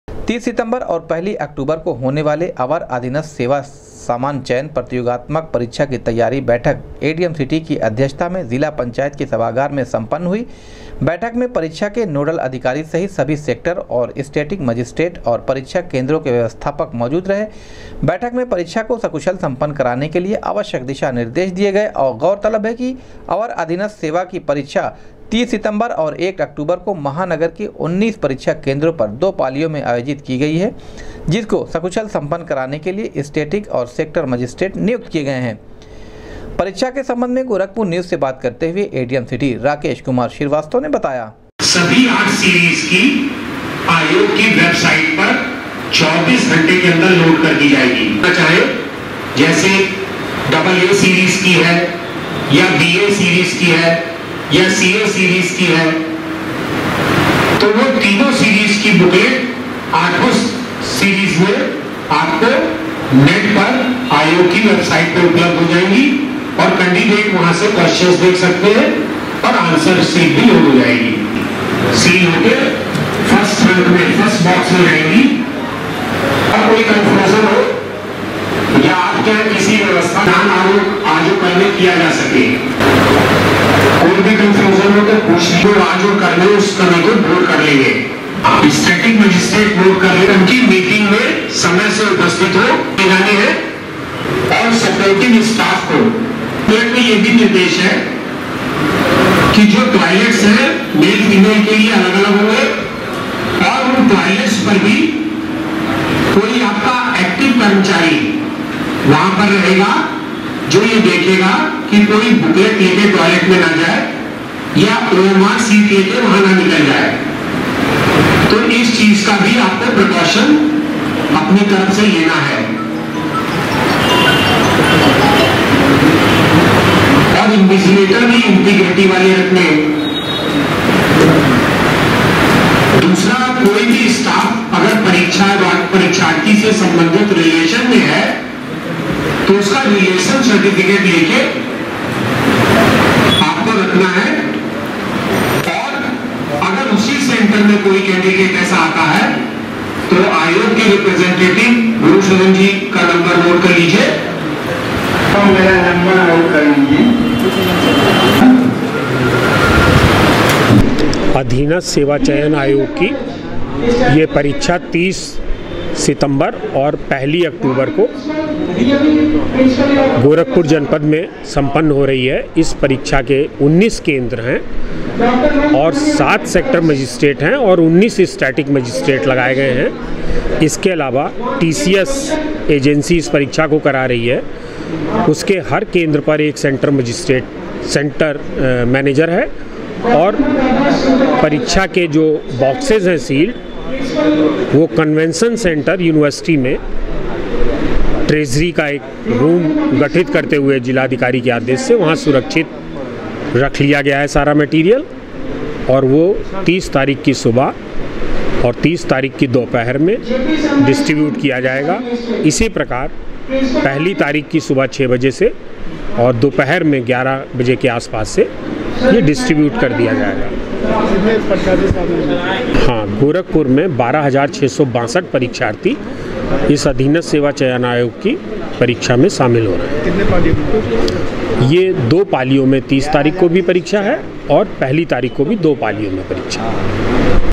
एडीएम सिटी की अध्यक्षता में सम्पन्न हुई अवर अधीनस्थ सेवा परीक्षा की तैयारी बैठक
एडीएम सिटी की अध्यक्षता में सम्पन्न हुई अवर अधीनस्थ सेवा परीक्षा की तैयारी बैठक, 30 सितम्बर और पहली अक्टूबर को 19 केंद्रों पर दो पाॅलियों में आयोजित होगी परीक्षा